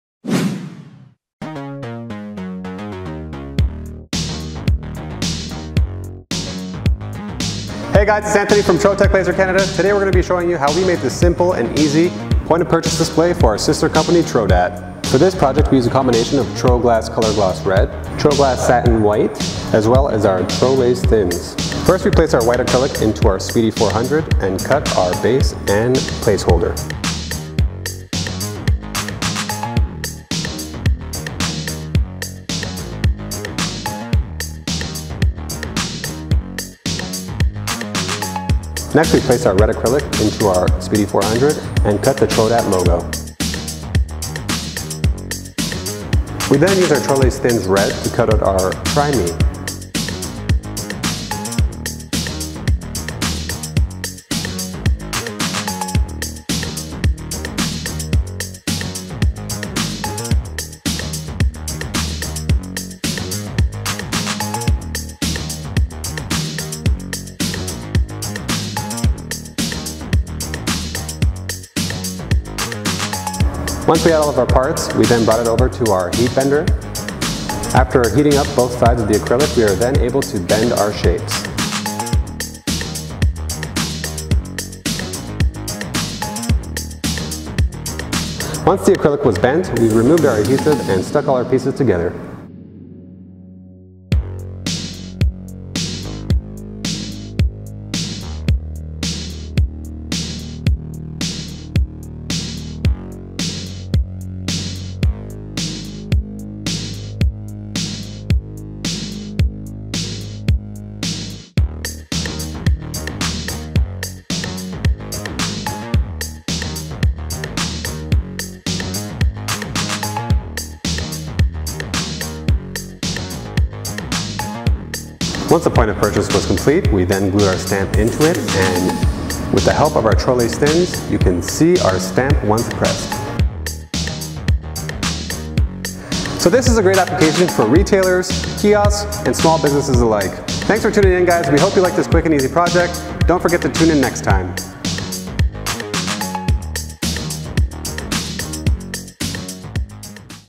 Trodat Stamp Display | Stamp POP Display | Laser Cut Acrylic
This week we're teaming up with our sister company Trodat Marking Canada to make an amazing laser cut acrylic POP display for their Printy Stamp.

We use our Speedy 400 laser cutting machine to cut clear, white and red TroGlass cast acrylic. We use out acrylic heat bender to shape our design and glue it together using our 3M adhesive.

This is a great laser cutting application for Point-of-purchase display that can be used by promotional companies, sign makers, industrial designs, and any small businesses that sell our stamps!

If you'd like to purchase either the TroGlass White and Red Acrylic, TroLase Metallics Mirrored Plastic, the Acrylic Heat Bender or the stamp rubber, please visit our consumables website at: